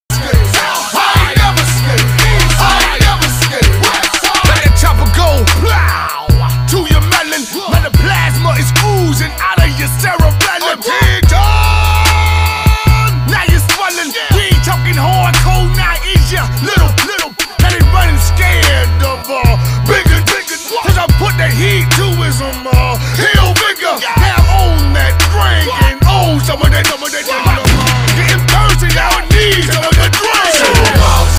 CRAZY!! shorts short viral funny funnyvideo viralfeeds reel love like reels youtubeshorts
This is a walking stick, and he can dance. probably has better moves than me. really cool for the kids to see.